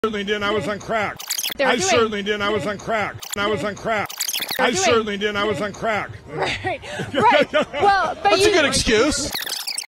My Pillow Guy, “I was on crack.”👀
Mike Lindell, the my pillow guy in an interview said he was on crack